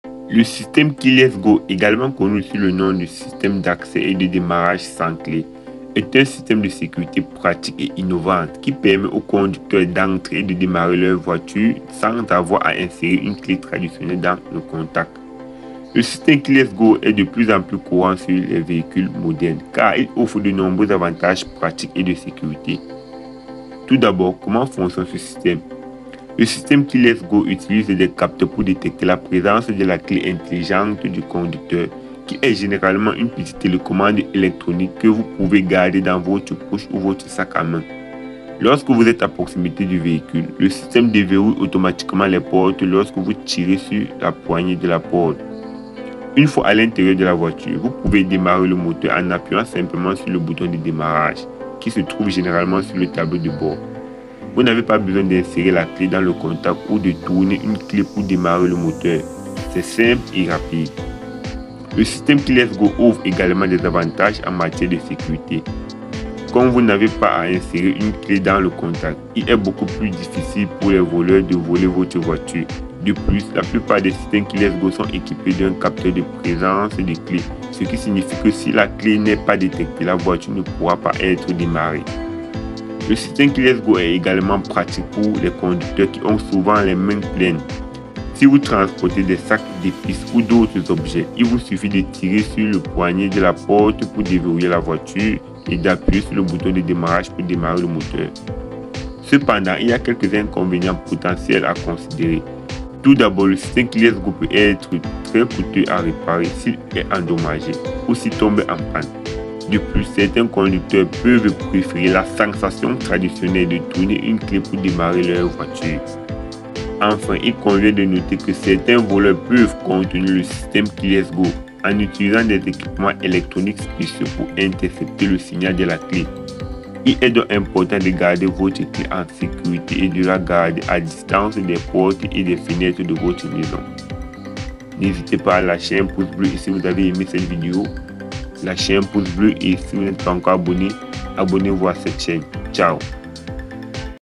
DÉMARRAGE VOITURE : Comment Fonctionne le SYSTÈME KEYLESS GO
Le système Keyless Go, également connu sous le nom de système d'accès et de démarrage sans clé, est un système de sécurité pratique et innovant qui permet aux conducteurs d'entrer et de démarrer leur voiture sans avoir à insérer une clé traditionnelle dans le contact.

🤓Si tu est intéressé pas une formation en ligne écrit moi en IB👉👇👇 ou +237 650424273

Idriss Foedje